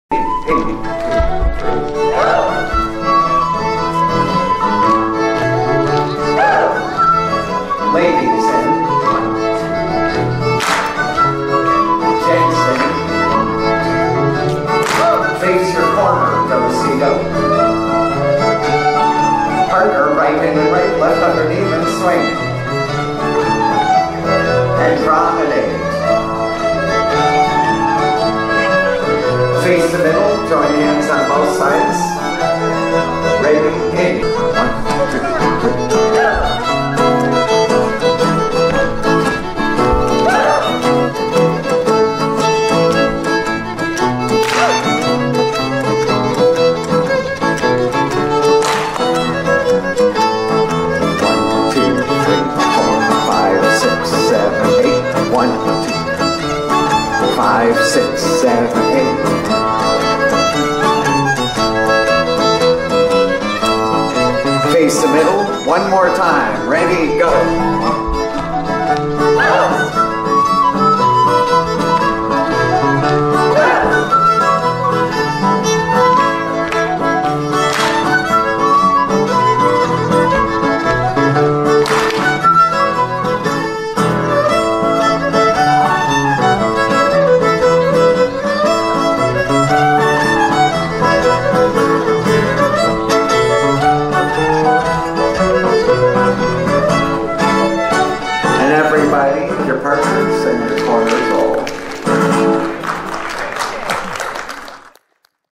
Circassian Circle
Circassian Circle done on Nov 10, 2018 in Proctors Black Box Theatre, Schenectady, NY.

Proctors Theatre, in association with the DanceFlurry Organization
and Old Songs Inc., presented an evening of community dancing in the Two Left Feet dance series.

Caller: Paul Rosenberg